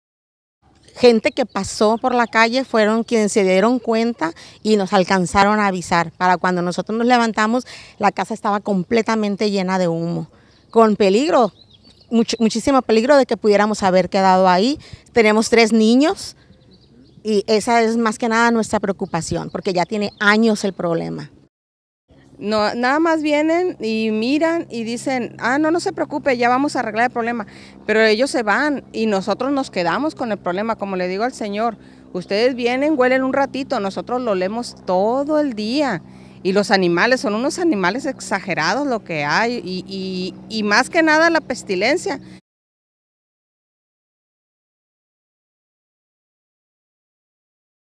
Piden limpiar casa incendiada en Industrial El Palmito
El pasado primero de enero se incendió una vivienda ubicada por la calle Cándido Aguilar en la colonia Industrial El Palmito.

Las autoridades se comprometieron a limpiar la vivienda donde habitaba un señor que acumulaba cosas el
cual afortunadamente en el incendio salió ileso.

Los vecinos están pidiendo a las autoridades que terminen de limpiar la casa ya que están llegando vagos y prenden fuego a lo que quedó en la vivienda además dicen que hay muchas ratas y el olor que sale del lugar es insoportable.